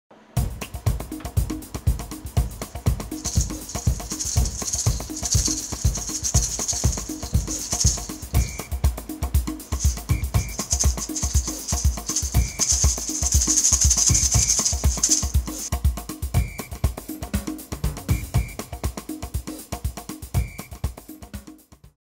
Crazy Cinco de Mayo Wooden Maracas
Take your party to the next level with our Wooden Maracas. Great for Cinco de Mayo, quinceañeras, beach parties, birthdays, and music events. And they’re actually handmade and hand painted in made in Mexico.

Please visit the listing in our Amazon store: